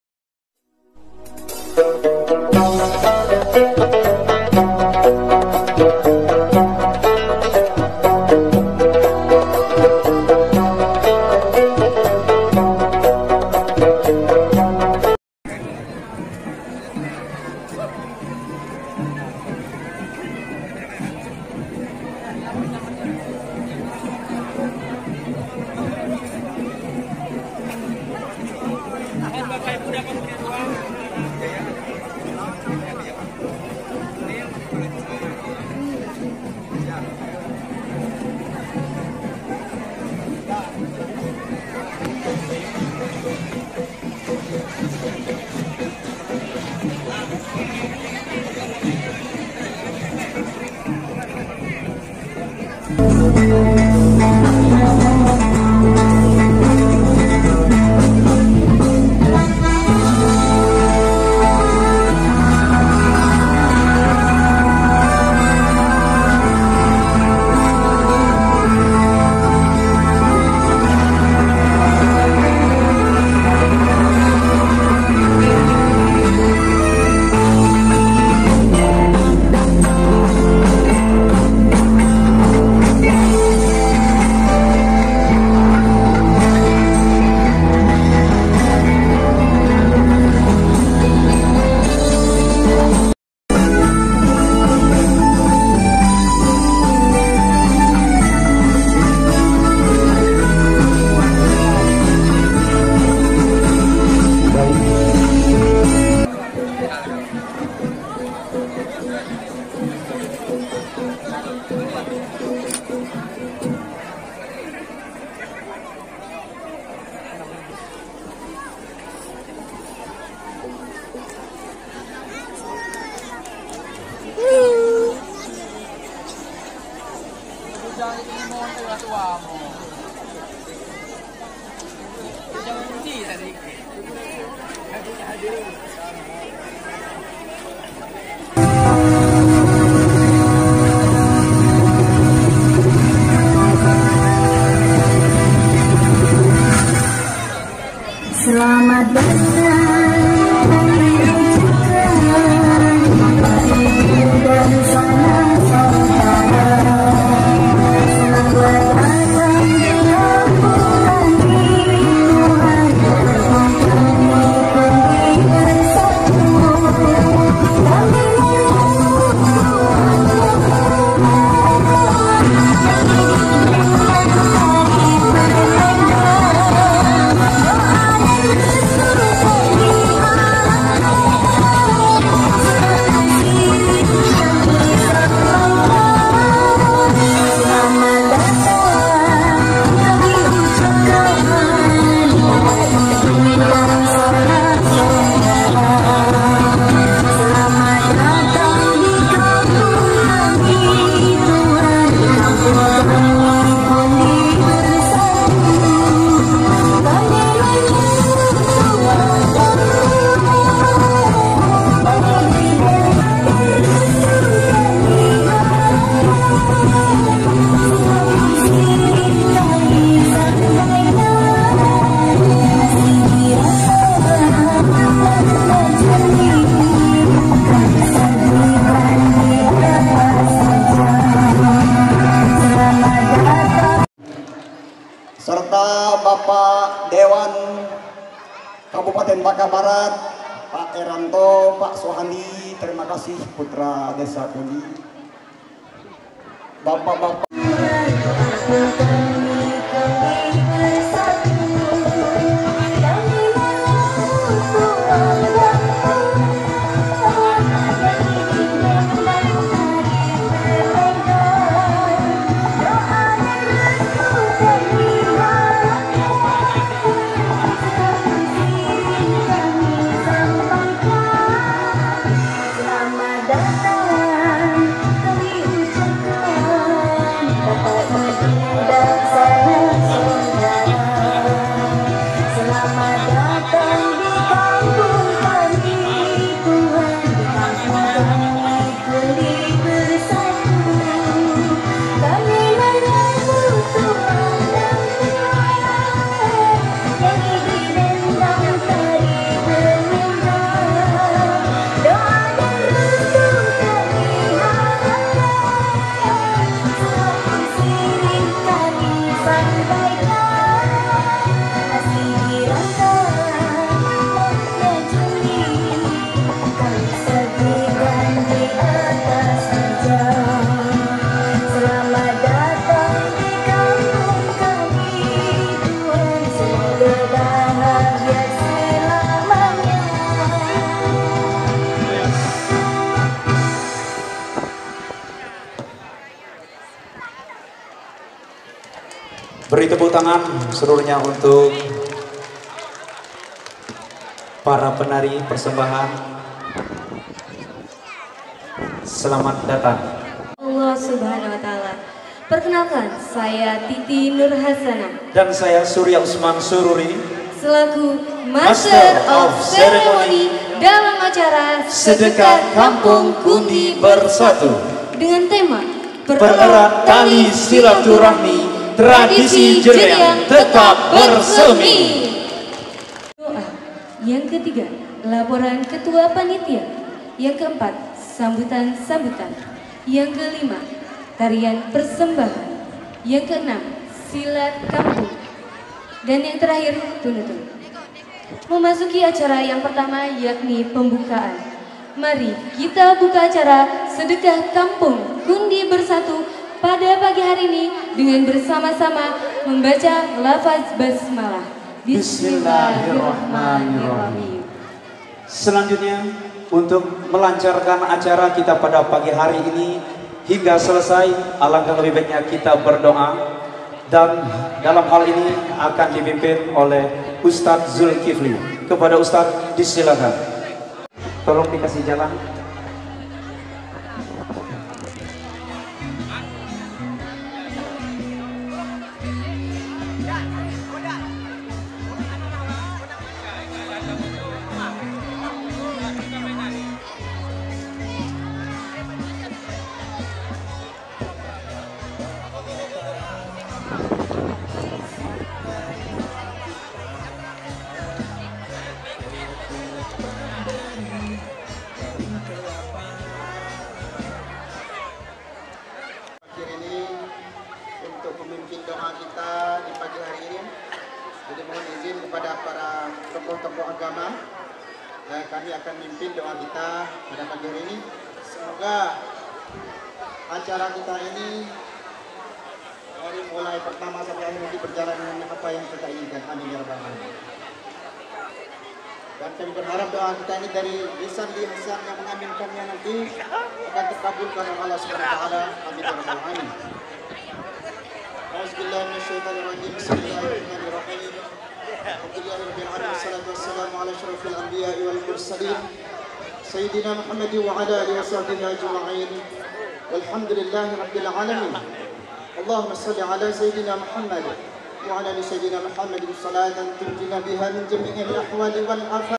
PENYAMBUTAN ARAK - ARAKAN BAPAK BUPATI DI BALAI DESA DENGAN TARI2AN #2
PENYAMBUTAN ARAK - ARAKAN BAPAK BUPATI DI BALAI DESA DENGAN TARI2AN
SETELAH ARAK2AN SEPANJANG KAMPUNG SAMPAI LAH DI BALAI DESA KUNDI BERSATU KEMUDIAN DI SAMBUT DENGAN TARI2AN

ACARA :
PENYAMBUTAN ROMBONGAN ARAK2AN BAPAK BUPATI, KHATAMAN, SUNATAN DAN SEMARANGAN DLL

WATU & LILOKASI :
MINGGU 03 SEPTEMBER 2023 BALAI DESA KUNDI BERSATU

KAMERA/ EDITOR : AMS
AD : RN